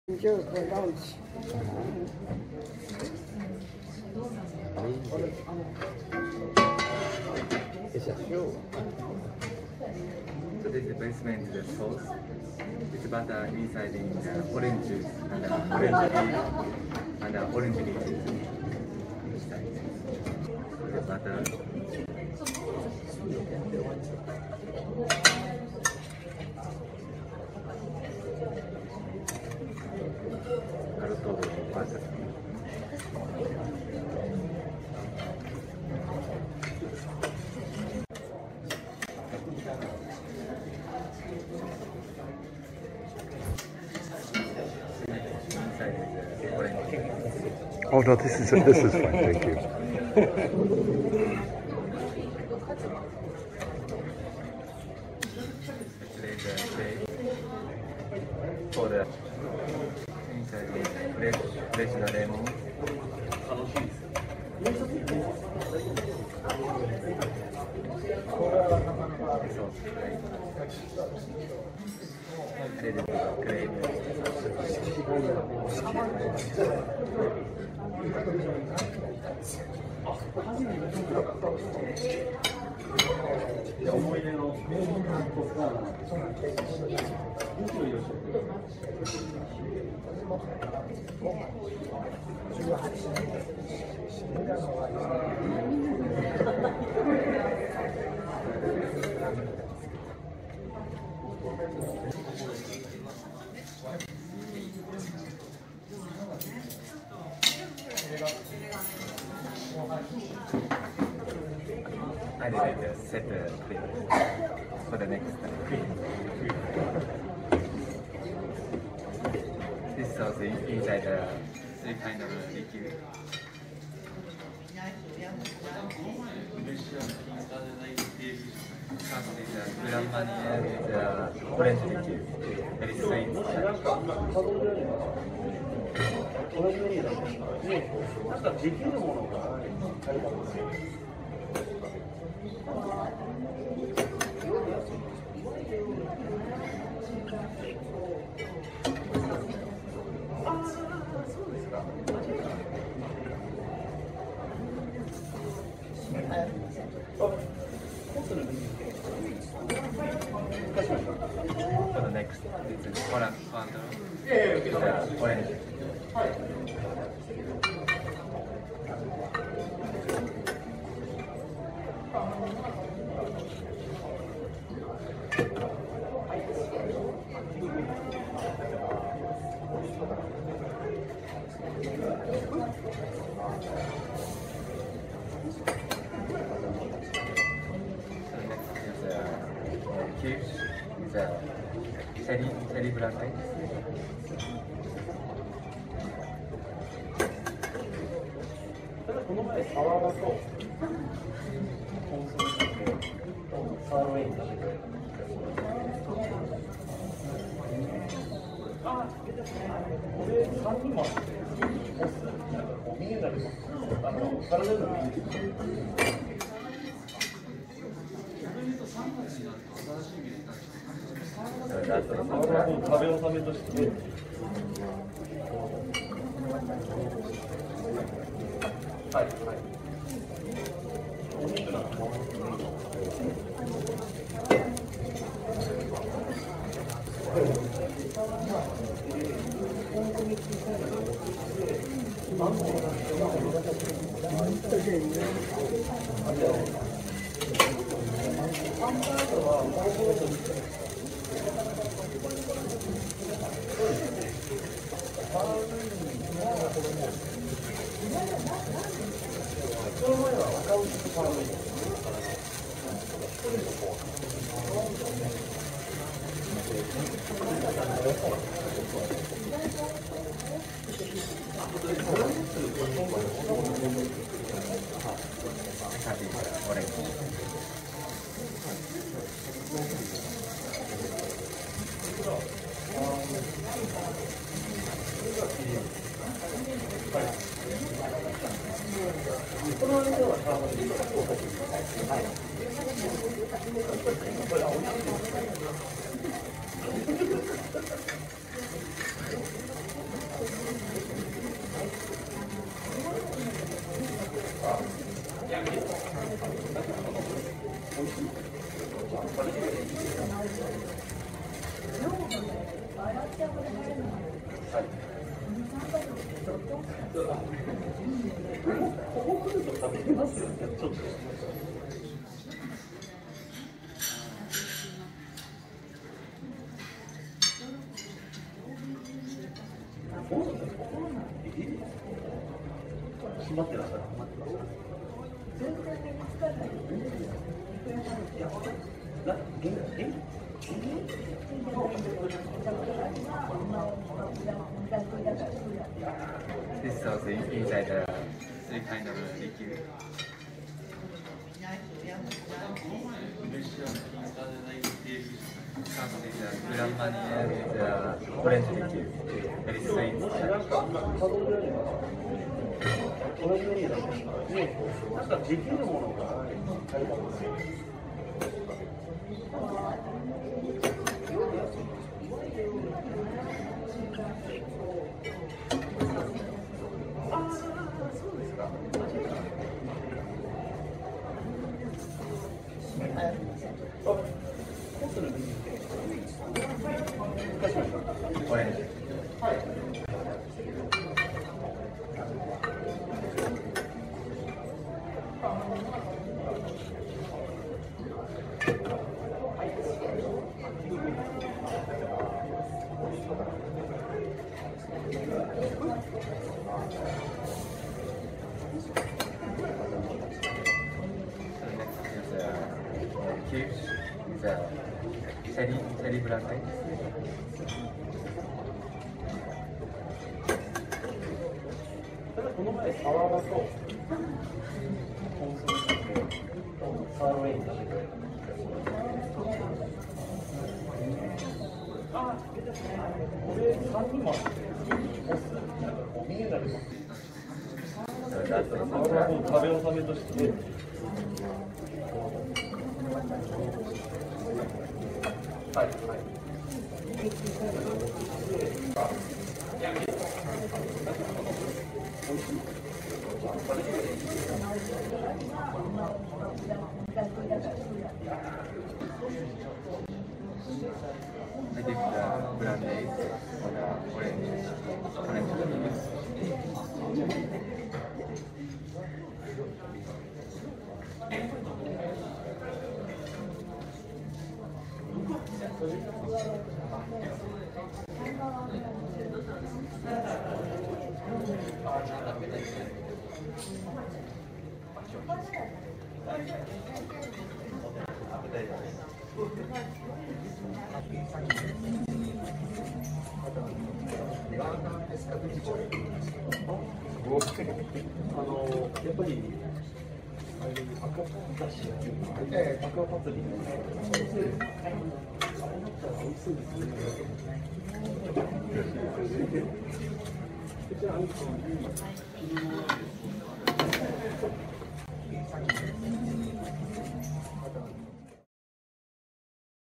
Crepe Suzette
Crepe Suzette made table side at Nouvelle Epoque in the Okura Hotal Tokyo.